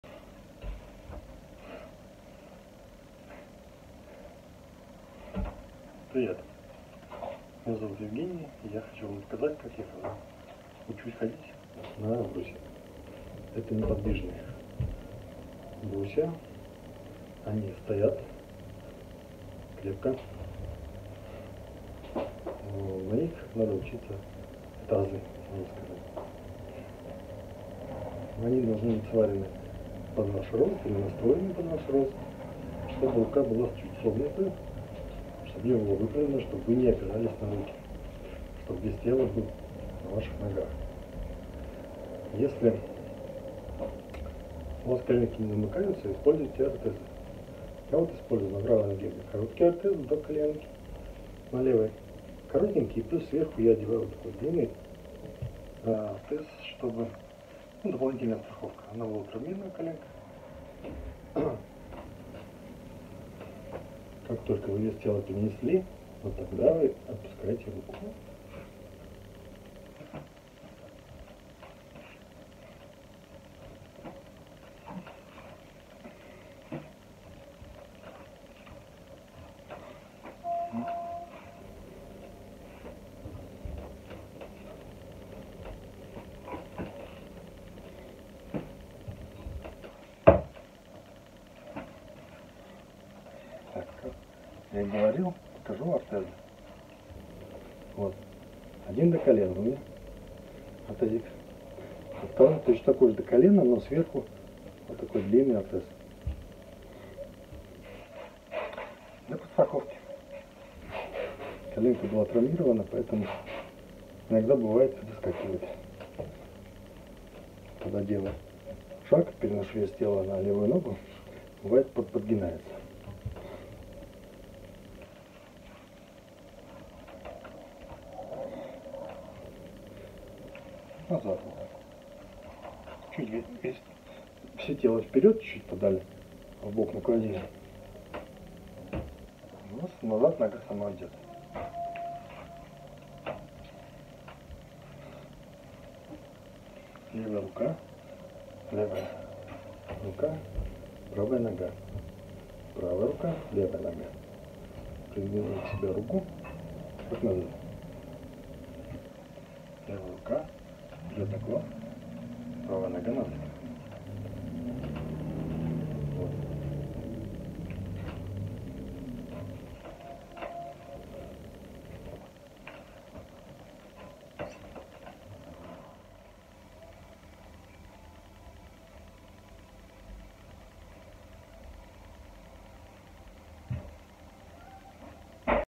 Спинальник. Грудной отдел. Учусь ходить с комментариями. Wheelchairman. T5671011 sci. With comments
Я рассказываю , как учусь ходить.

I tell you how I study to walk.